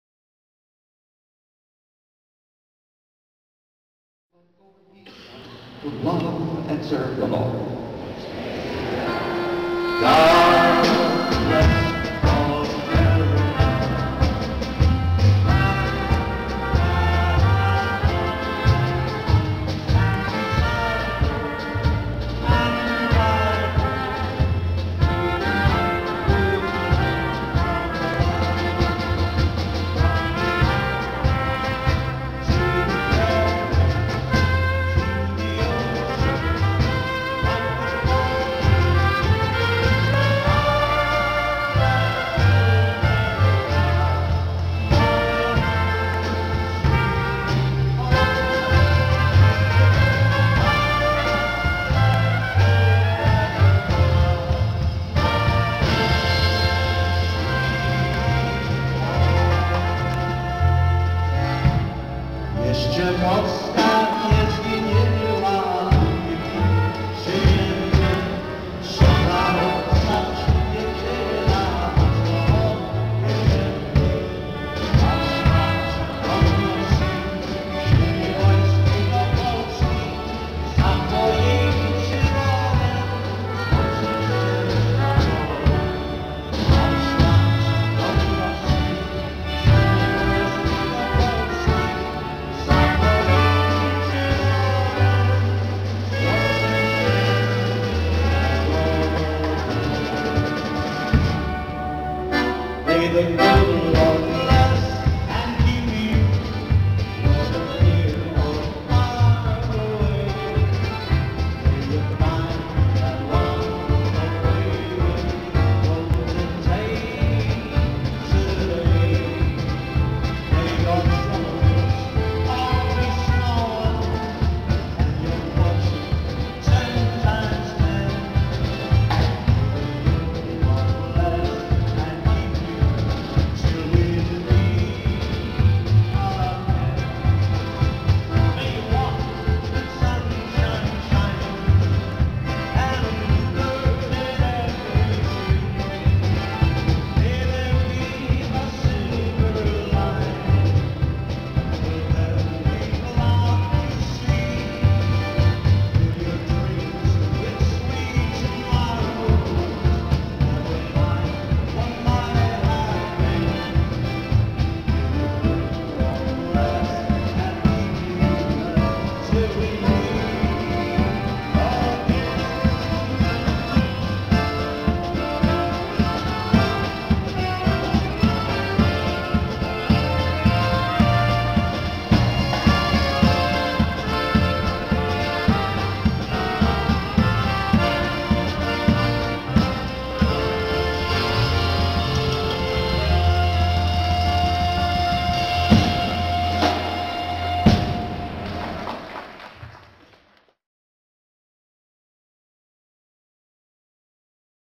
Saint Adalbert Basilica - Anniversary Kickoff Mass featuring the Buffalo Touch
Saint Adalbert Basilica is entering its 125th year in Buffalo...its kicked off this year long celebration by hosting a Polka Mass featuring the Buffalo Touch